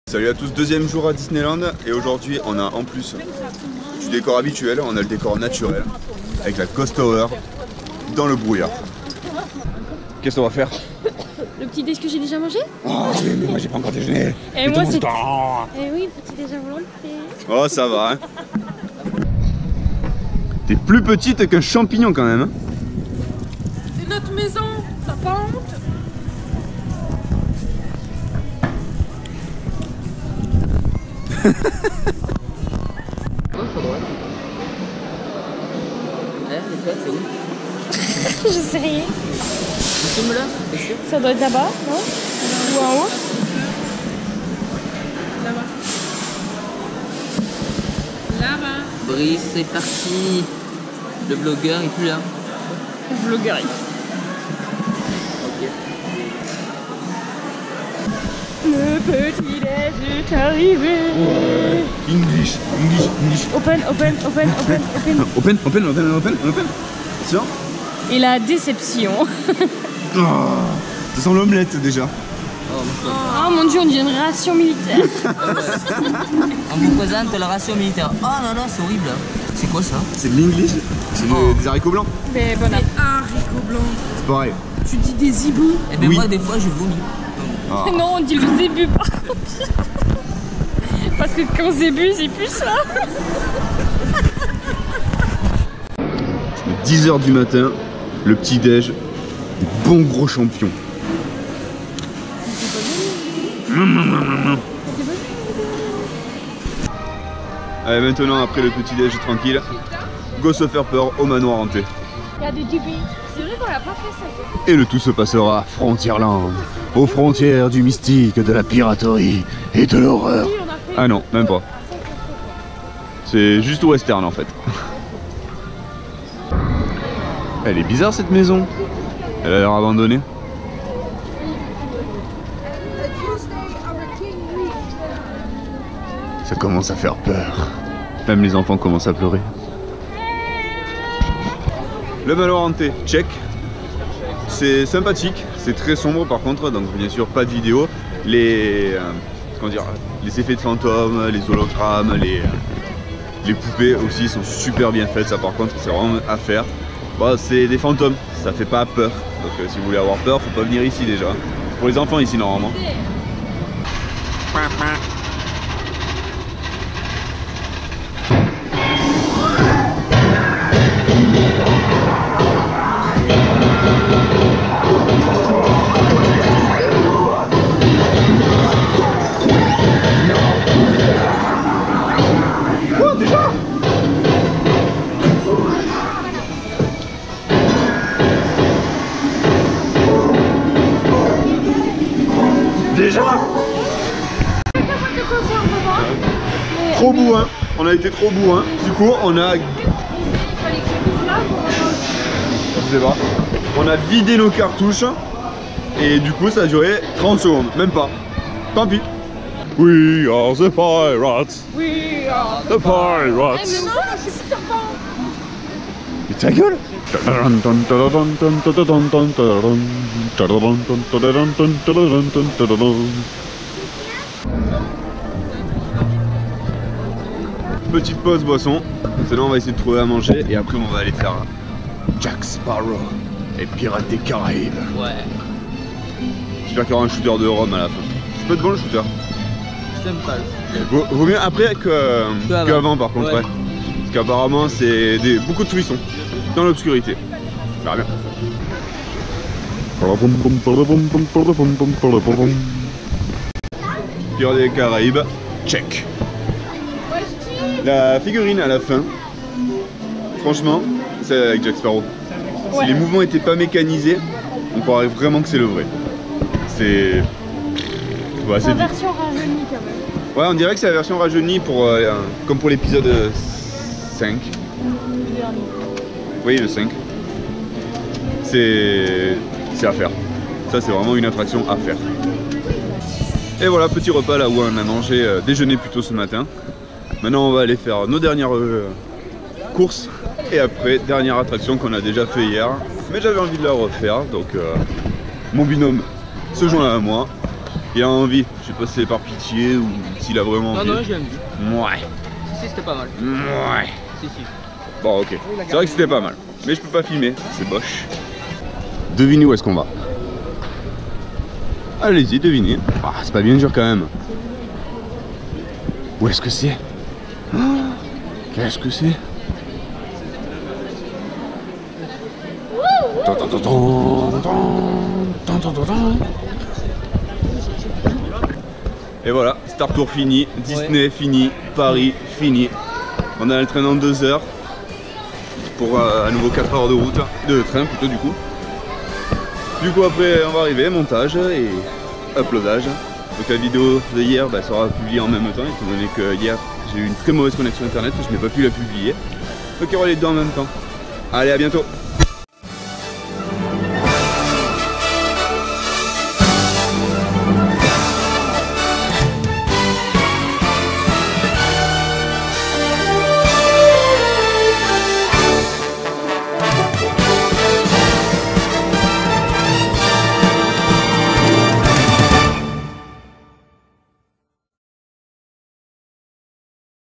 DISNEYLAND JOUR 2
Deuxième et dernier jour à Disneyland Paris. Quoi qu'il ce passe, j'en garderai de bon souvenir, JE - ME - ÉCLATÉ.